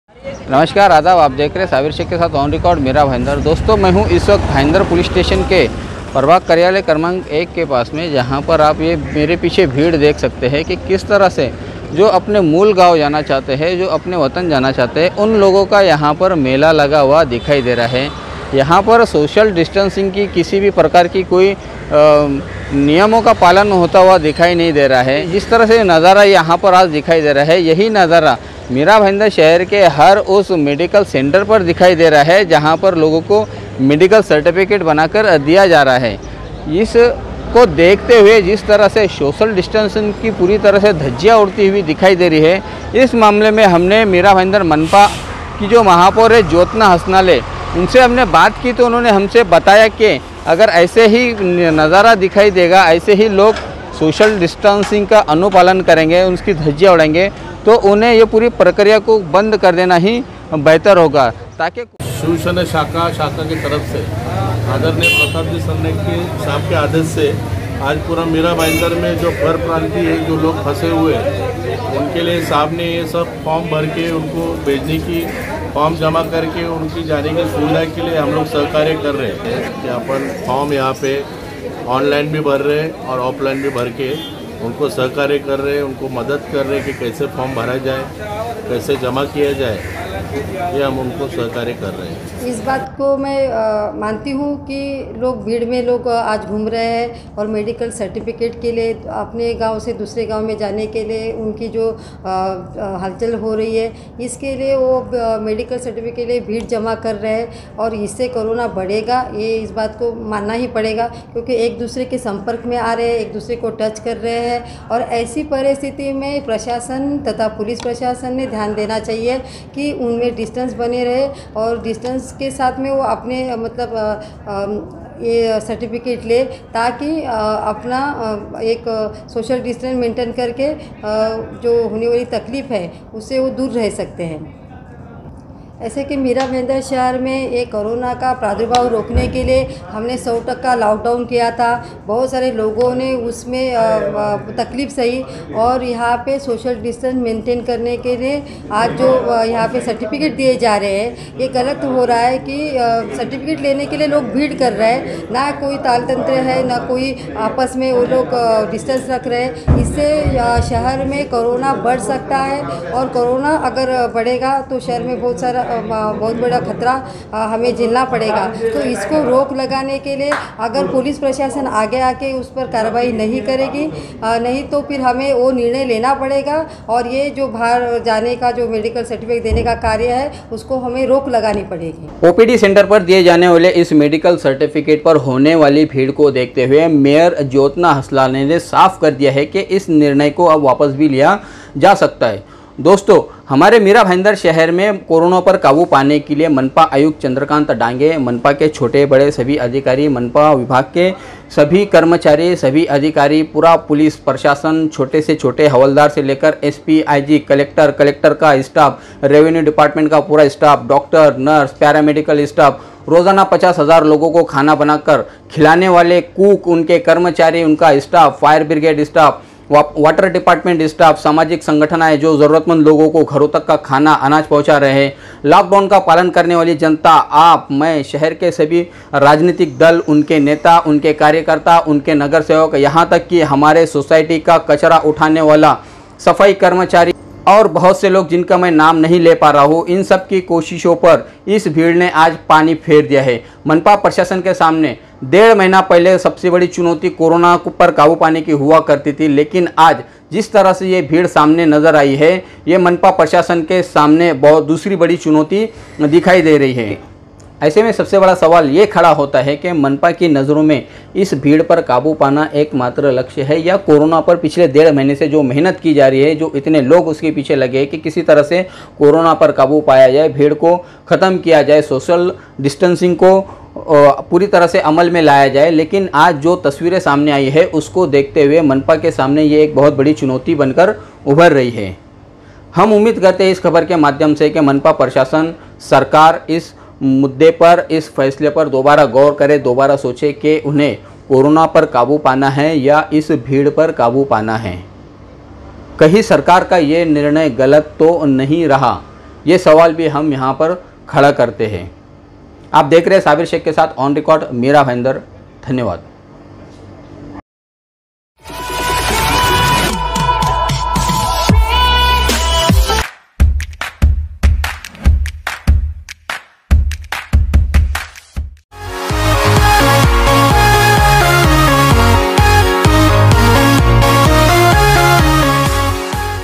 मेडिकल सर्टिफिकेट लेने के दौरान उमड़ा जन सैलाब
मेडिकल सर्टिफिकेट लेने के दौरान सामूहिक दूरी की उड़ गयी धज्जिया
सोशल डिस्टेन्सिंग का होगयी फुर्ररर..
मेडिकल सर्टिफिकेट देने का निर्णय हो सकता रद्द